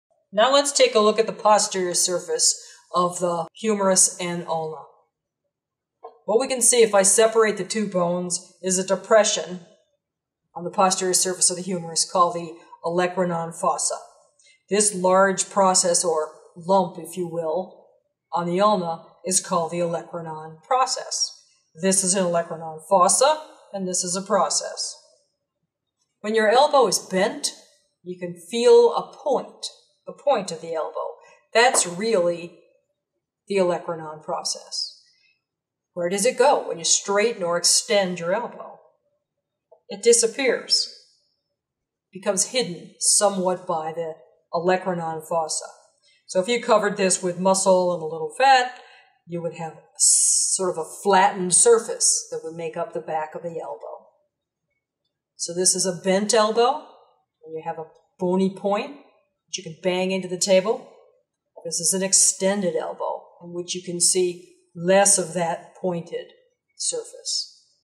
Elbow Bones
The bones of the elbow joint are examined.